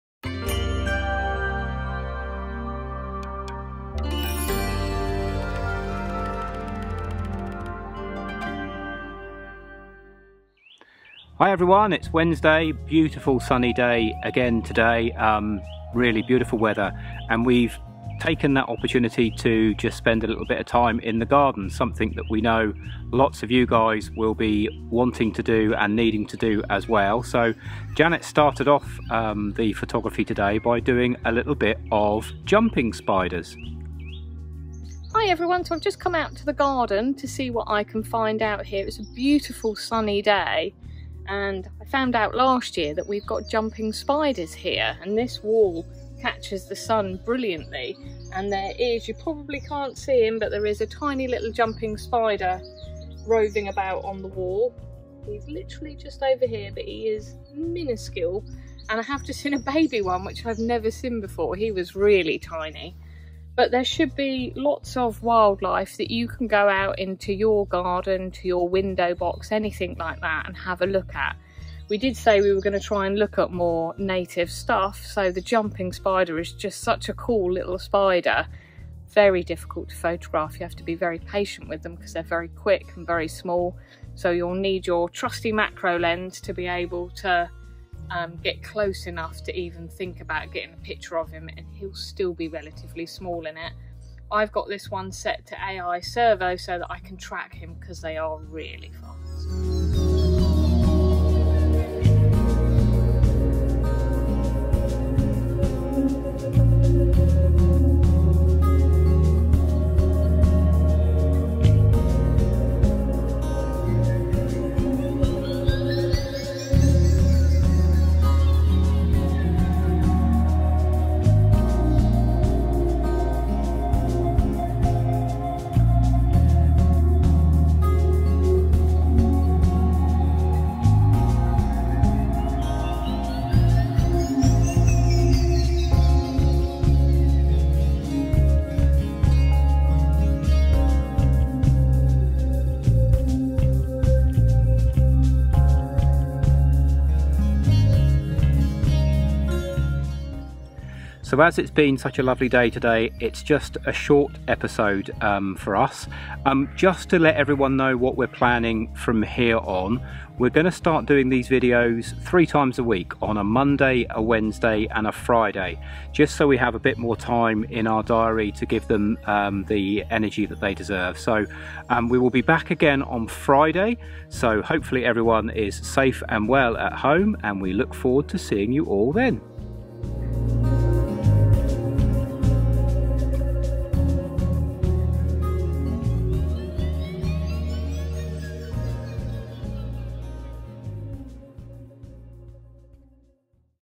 Tales From The Knowsley Bush - Day 14
With todays beautiful weather, we have mostly been in the garden.